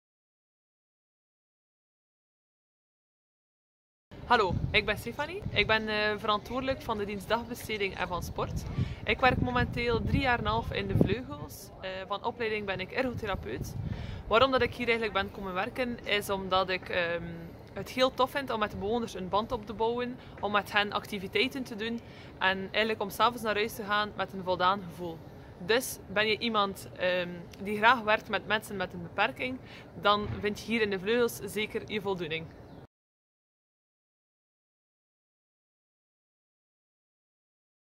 Jobhappening @deVleugels - Stefanie
Jobhappening @deVleugels. Geboeid en nieuwsgierig naar een job in de zorg? Beschik je over mensenkennis, creativiteit en zorg je voor sfeer? Heb je zin voor samenwerking en initiatief? Dan is werken als ergotherapeut zoals Stefanie misschien wel iets voor jou. 👉Delen vinden we tof!